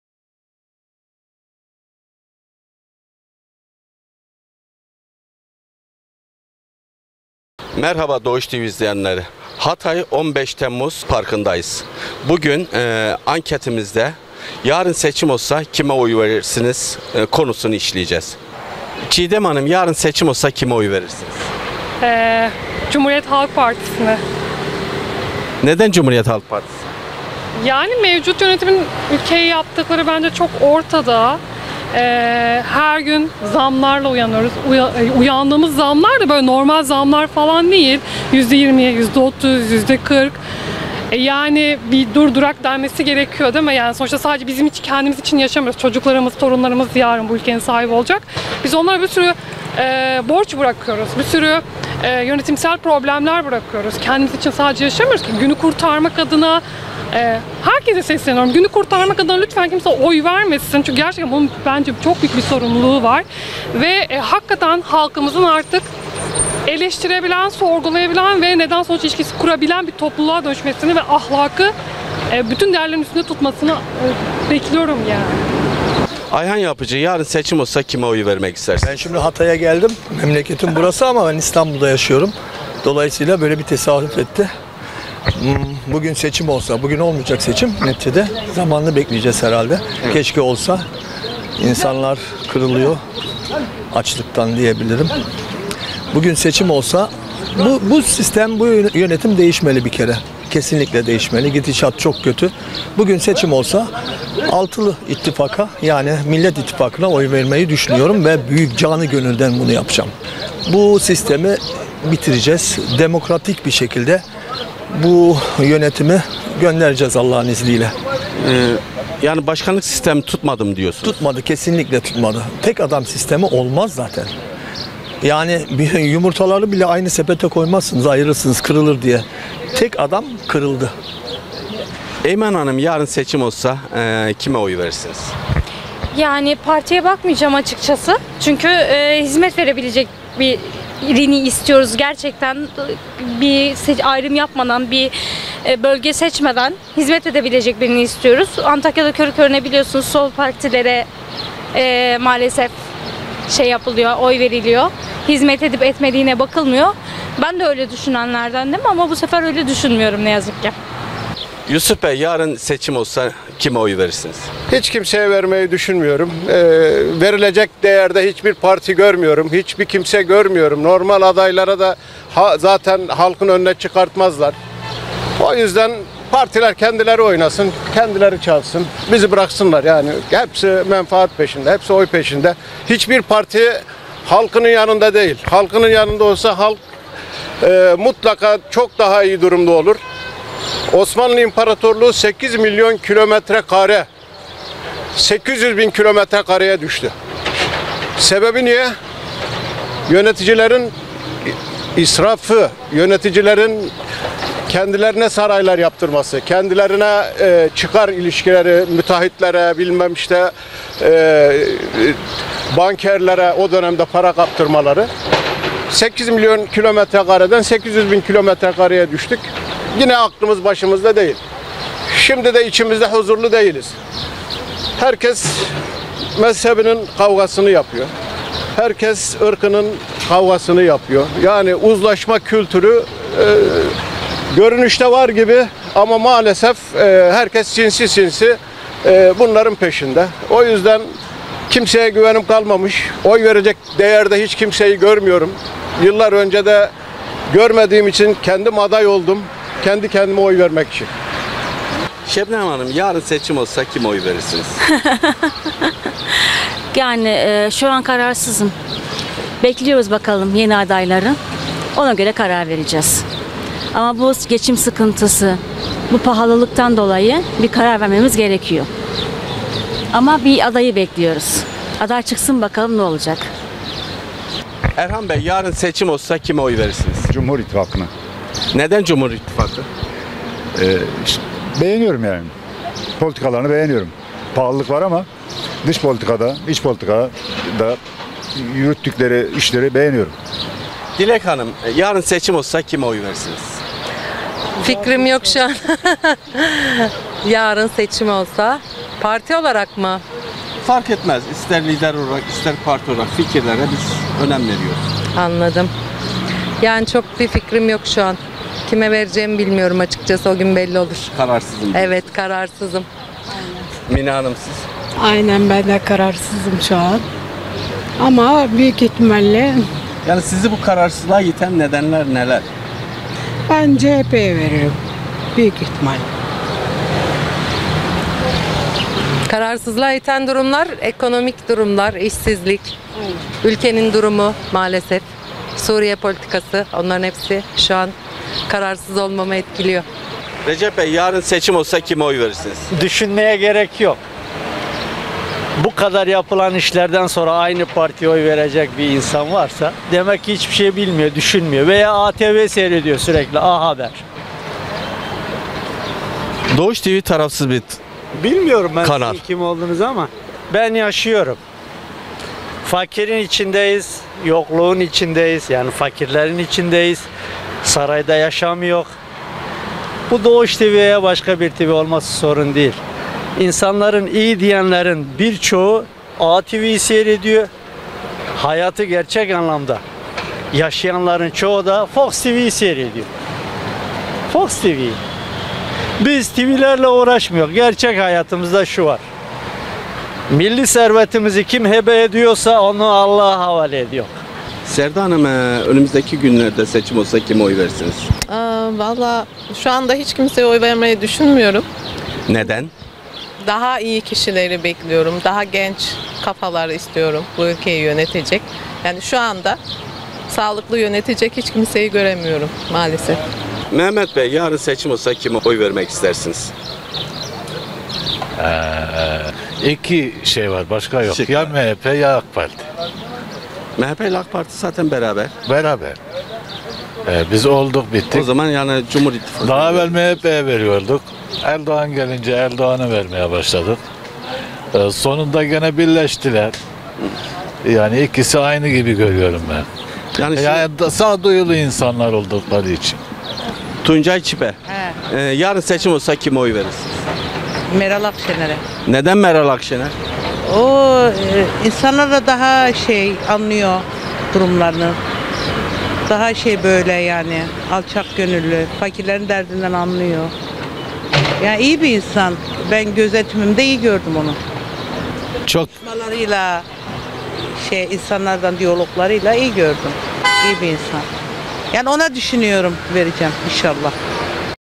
Doğuş TV Ailesi Olarak Halkın Nabzını Tutmaya Devam Ediyoruz.
Halkımıza Yakın Zamanda Seçim Olursa Kimi Destekleyeceğini Sorduk.